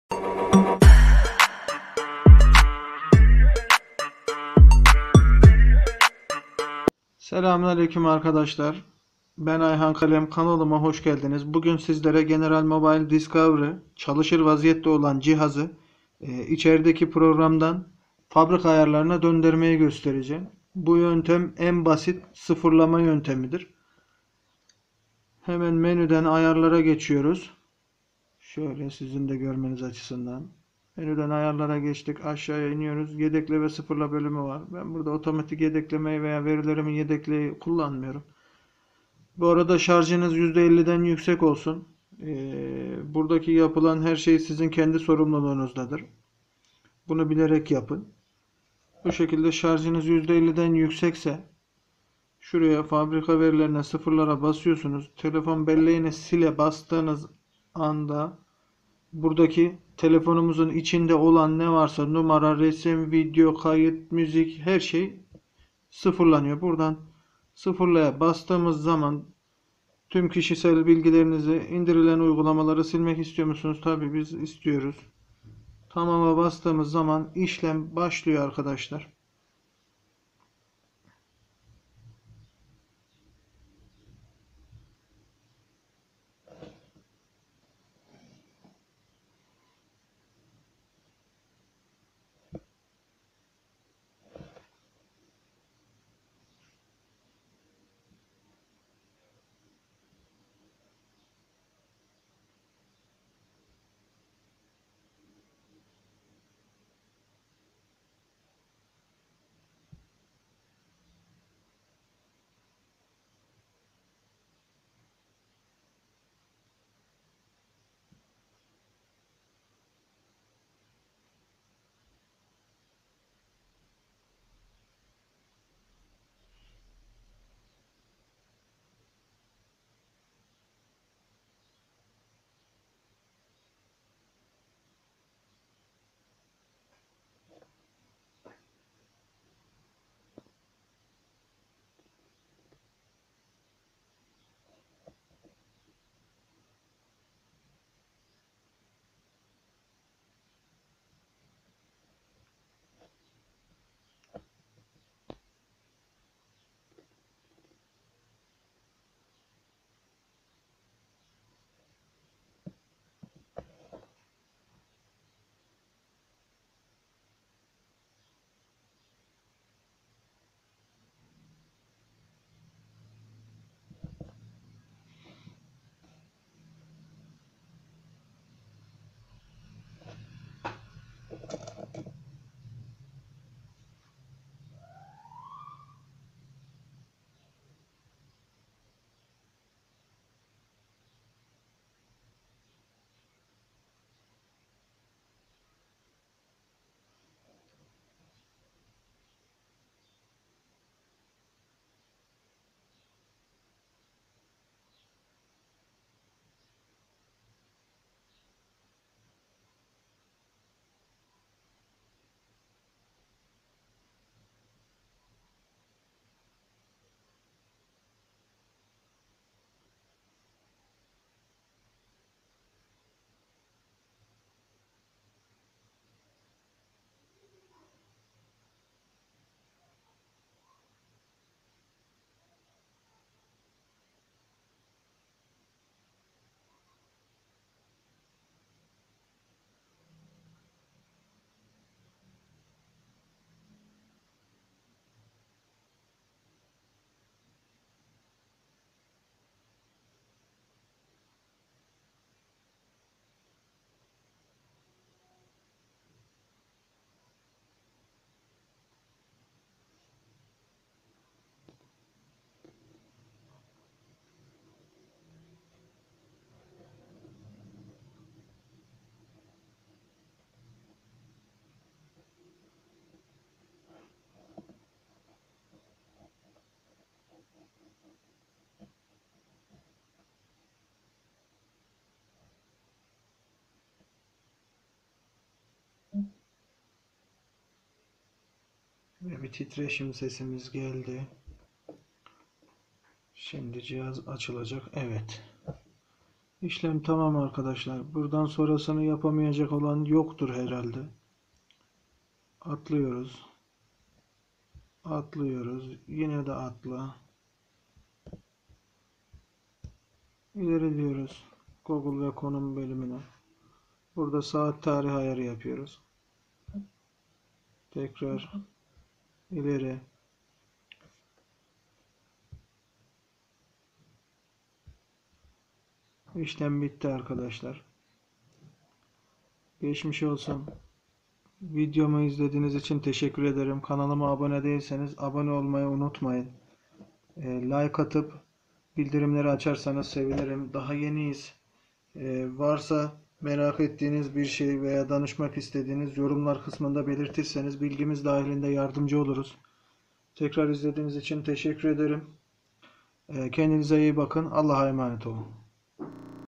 Format Atma General Mobile Discovery En Kolay Yoldan Sıfırlama (Tüm Cihazlar İçin Geçerli )
Selamın Aleyküm Dostlarım
General Mobile Discovery  cihazınız haddinden fazla donmaya başladı ise format atmayı en kolay yoldan sizlere göstermeye çalıştım.
Umarım beğenmişsinizdir...
Herkese kolay gelsin
LÜTFEN DESTEK OLUN BEĞENİN VARSA SORUNUZ SORUNUNUZ SORUN BİLGİMİZ DAHİLİNDE YARDIMCI OLMAYA ÇALIŞALIM